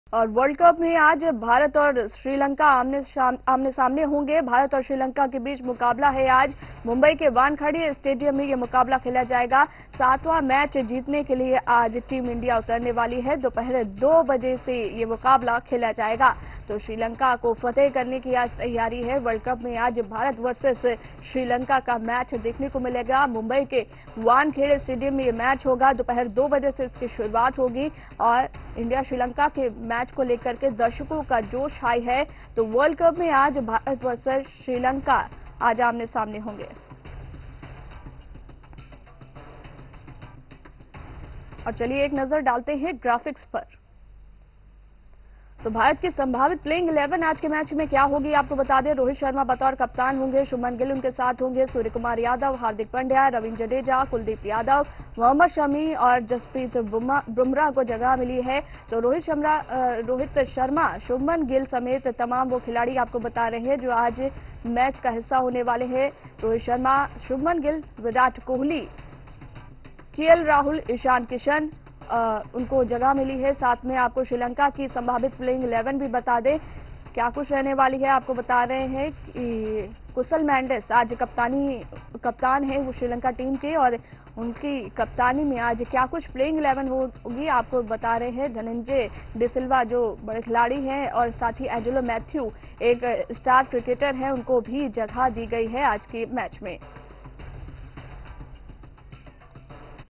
भारत और श्रीलंका के बीच आज मुकाबला मुंबई के वानखेड़े स्टेडियम में खेला जाएगा...
भारत आज विश्‍वकप में अपना सातवां मैच खेलने जा रहा है. मुंबई के वानखेड़े स्टेडियम में भारत का मुकाबला आज श्रीलंका के साथ होगा...